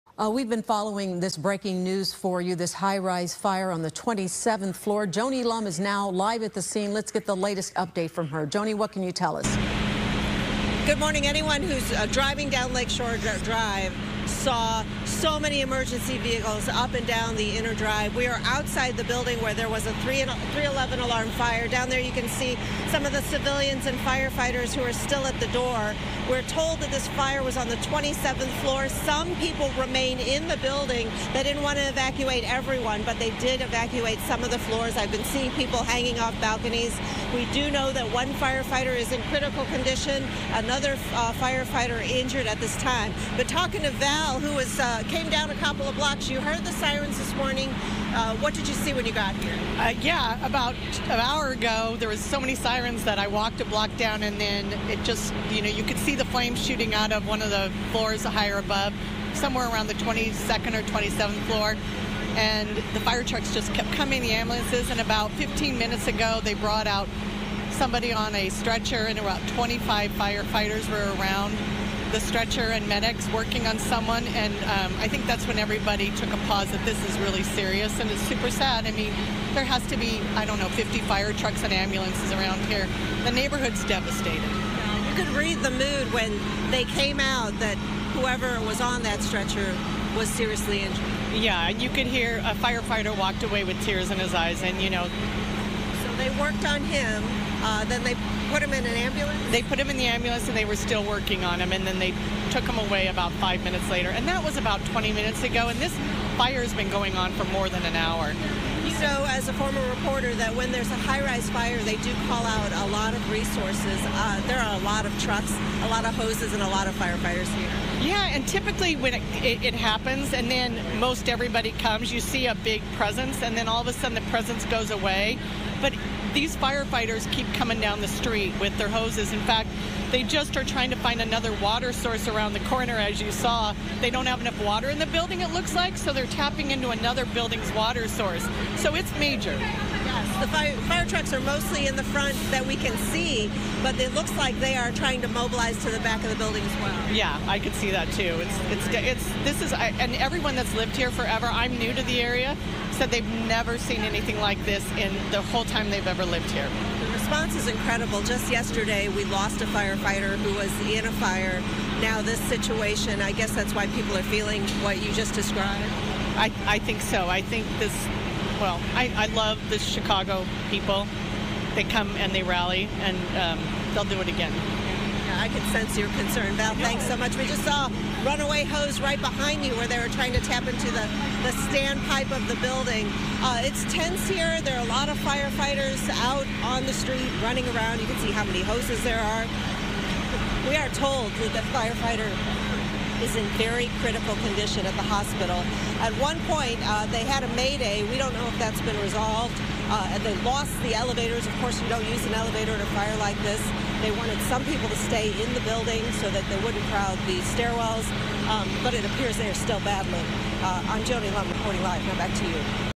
Chicago firefighter in critical condition after battling multi-alarm fire at highrise on Lake Shore
One firefighter is in critical condition and at least one other first responder was injured following a multi-alarm fire at a highrise in Gold Coast. Just yesterday, a Chicago firefighter was killed on the job at a fire on the South Side.